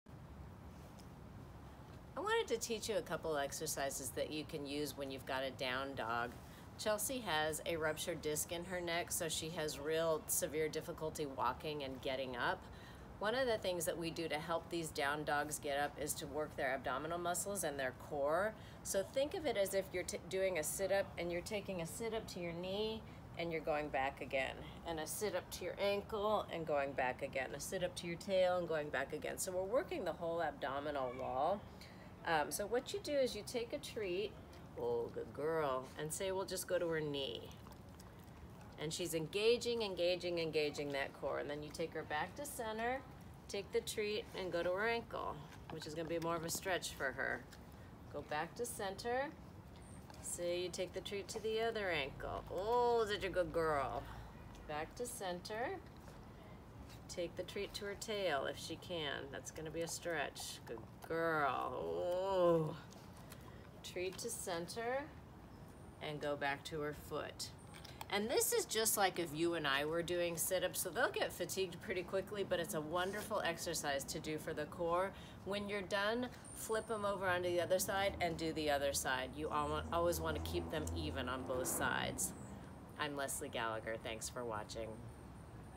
Lying down crunches with Chelsea - At Home Exercise Series
Lying down crunches is one of two exercises focused on core strength, particularly for a "down" dog.